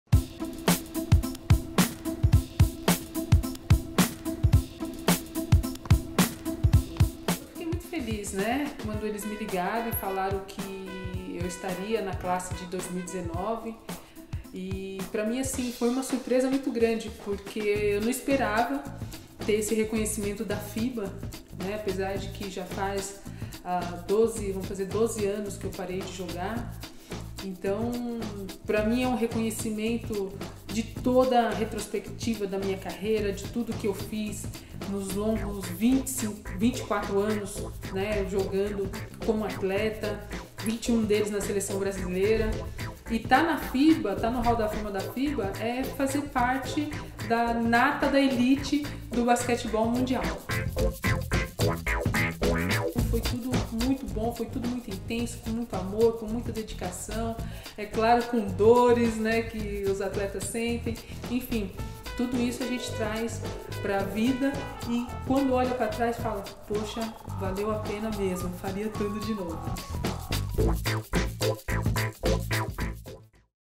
Janeth Arcain fala sobre Hall da Fama do basquete mundial
Um dos maiores nomes do basquete falou com o Terra sobre a seleção para a classe de 2019 do Hall da Fama da Federação Internacional de Basquete (Fiba).